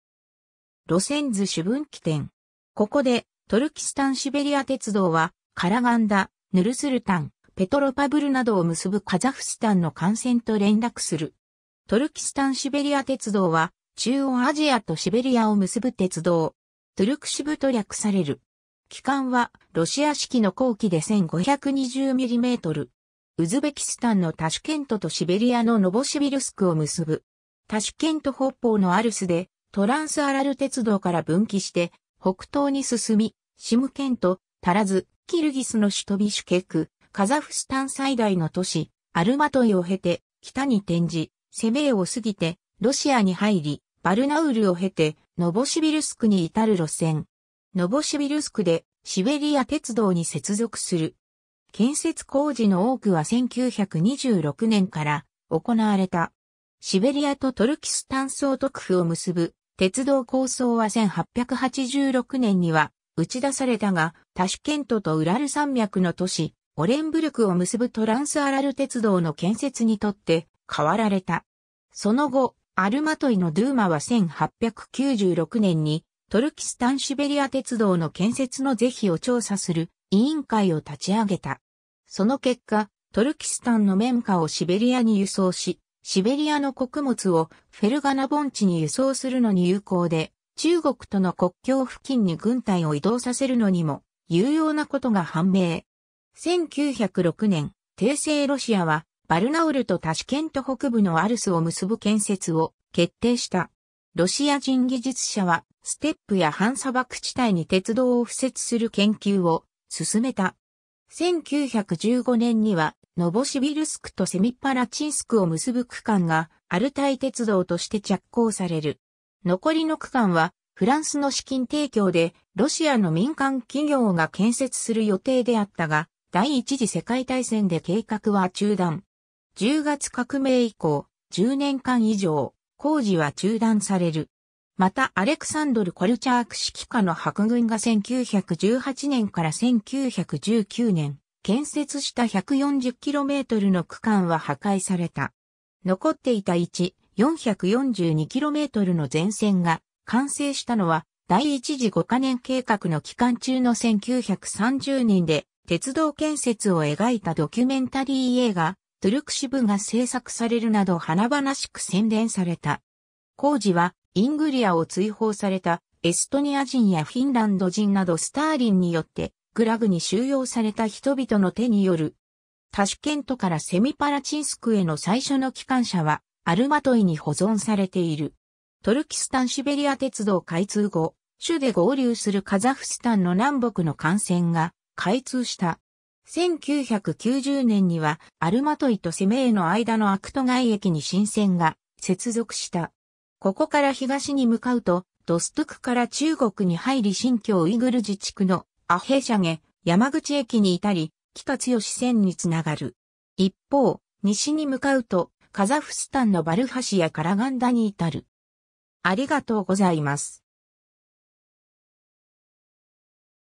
トルキスタン・シベリア鉄道
路線図 シュ分岐点。
ここでトルキスタン・シベリア鉄道はカラガンダ、ヌルスルタン、ペトロパブルなどを結ぶカザフスタンの幹線と連絡する。
 トルキスタン・シベリア鉄道（ロシア語: Туркестано-Сибирская магистраль）は、中央アジアとシベリアを結ぶ鉄道。
トゥルクシブ（Турксиб）と略される。
軌間はロシア式の広軌で1,520mm。
ウズベキスタンのタシュケントとシベリアのノヴォシビルスクを結ぶ。
 タシュケント北方のアルス（アルィーシ）で、トランス・アラル鉄道から分岐して、北東に進み、シムケント、タラズ、キルギスの首都ビシュケク、カザフスタン最大の都市・アルマトイを経て北に転じ、セメイを過ぎてロシアに入りバルナウルを経てノヴォシビルスクに至る路線。
ノヴォシビルスクでシベリア鉄道に接続する。
建設工事の多くは1926年から行われた。
 シベリアとトルキスタン総督府を結ぶ鉄道構想は1886年には打ち出されたが、タシュケントとウラル山脈の都市・オレンブルクを結ぶトランス・アラル鉄道の建設に取って代わられた。
その後、アルマトイのドゥーマは1896年に、トルキスタン・シベリア鉄道の建設の是非を調査する委員会を立ち上げた。
その結果、トルキスタンの綿花をシベリアに輸送し、シベリアの穀物をフェルガナ盆地に輸送するのに有効で、中国との国境付近に軍隊を移動させるのにも有用なことが判明。
1906年、帝政ロシアはバルナウルとタシュケント北部のアルスを結ぶ建設を決定した。
ロシア人技術者は、ステップや半砂漠地帯に鉄道を敷設する研究を進めた。
1915年にはノヴォシビルスクとセミパラチンスク（今日のセメイ）を結ぶ区間が「アルタイ鉄道」として着工される。
残りの区間はフランスの資金提供でロシアの民間企業が建設する予定であったが、第一次世界大戦で計画は中断。
十月革命以降、10年間以上、工事は中断される。
またアレクサンドル・コルチャーク指揮下の白軍が1918年から1919年建設した140kmの区間は破壊された。
 残っていた1､442kmの全線が完成したのは第一次五ヶ年計画の期間中の1930年で、鉄道建設を描いたドキュメンタリー映画「トゥルクシブ」が製作されるなど華々しく宣伝された。
 工事は、イングリアを追放されたエストニア人やフィンランド人などスターリンによってグラグに収容された人々の手による。
タシュケントからセミパラチンスクへの最初の機関車は、アルマトイに保存されている。
 トルキスタン・シベリア鉄道開通後、シュで合流するカザフスタンの南北の幹線（カラガンダ、ヌルスルタン、ペトロパブルなどを結ぶ）が開通した。
 1990年には、アルマトイとセメイの間のアクトガイ駅に新線が接続した。
ここから東に向かうと、ドストゥク（ドルージュバ）から中国に入り新疆ウイグル自治区の阿拉山口駅に至り、北疆線に繋がる。
一方、西に向かうとカザフスタンのバルハシやカラガンダに至る。
 （主要駅のみ掲載）